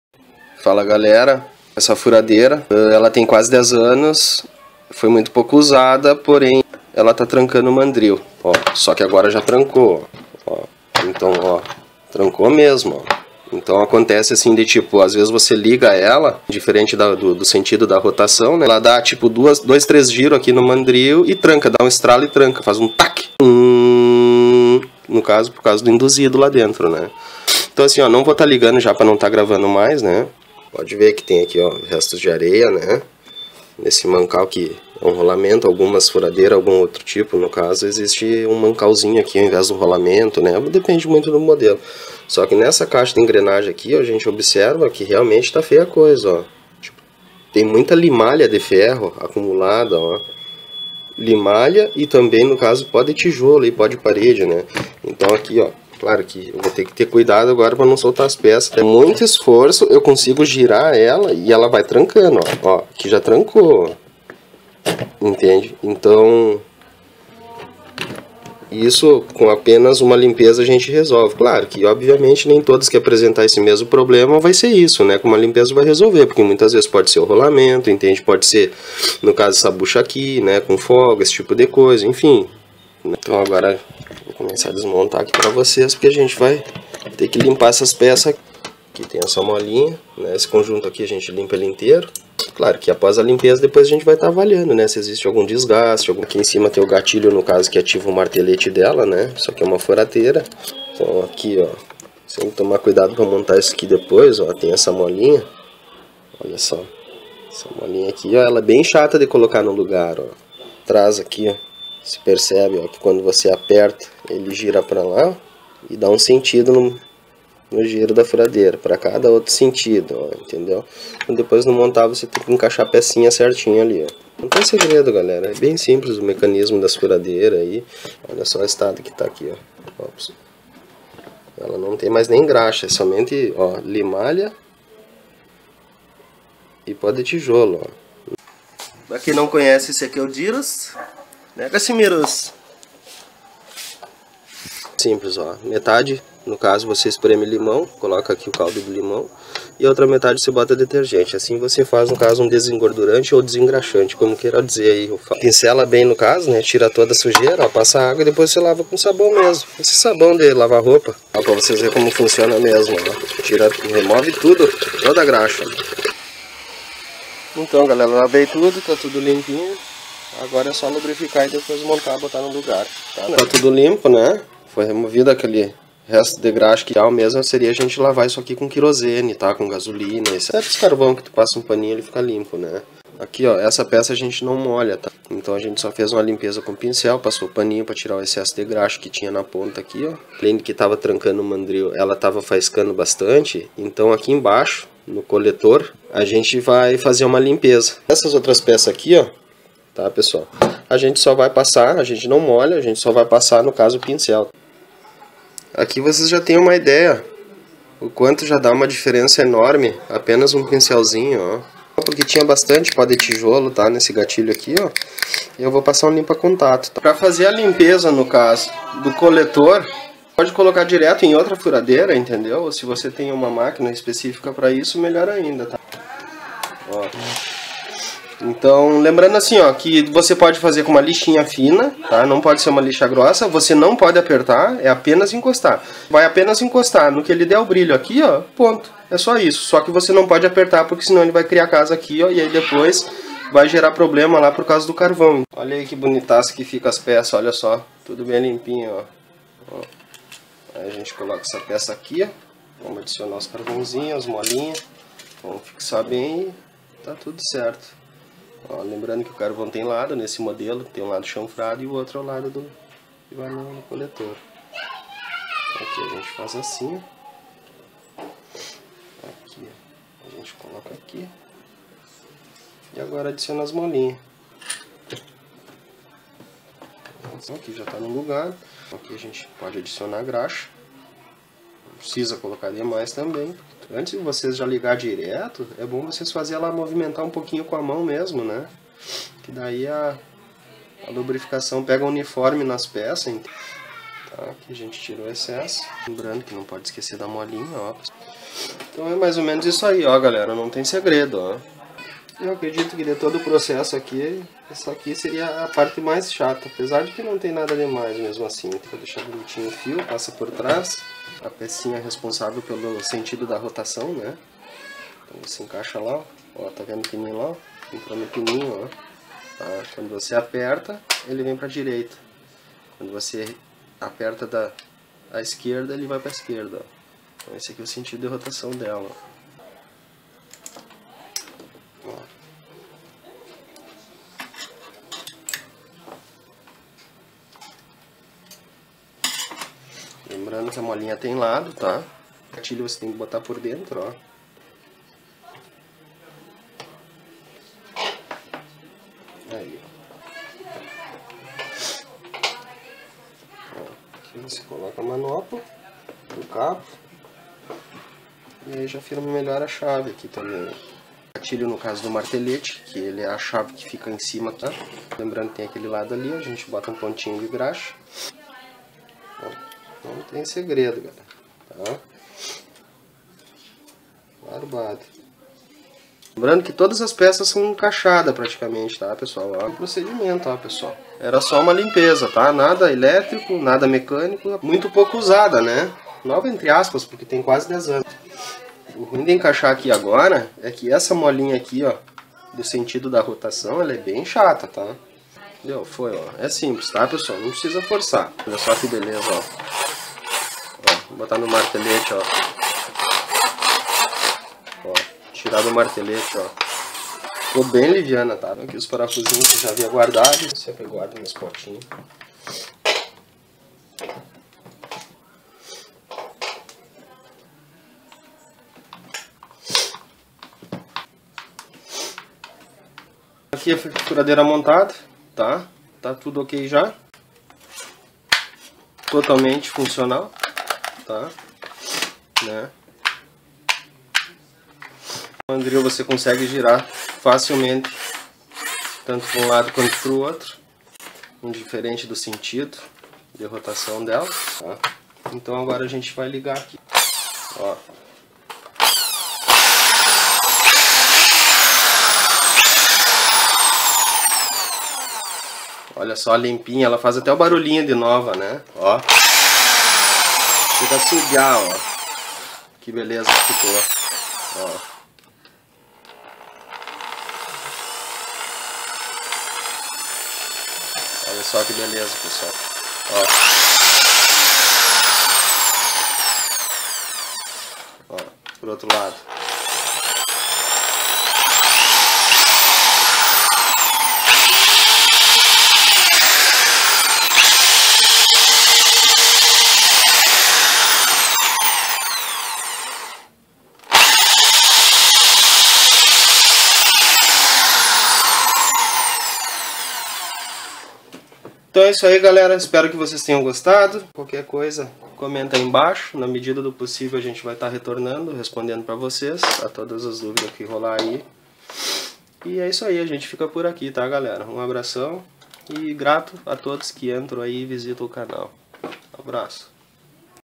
Defeito Furadeira Black&Decker TM650-B2 trancando mandril
De forma simples explica: Dicas e macetes para recuperar furadeira que tranca o mandril.

IMPORTANTE:
Era  sujeira que se acumulou durante quase 10 anos, indiferente do sentido da rotação 'Trancava'...

O intuito é ajudar quem passa pelo mesmo ou parecido problema...
NÃO havia defeitos elétrico nem mecânico!

OBS: por mais parecido que seja os sintomas defeituosos, deve sempre observar se existem peças com desgastes e ou rolamentos e buchas  estragados, no meu caso apenas limpar resolveu.

Grato pela visita.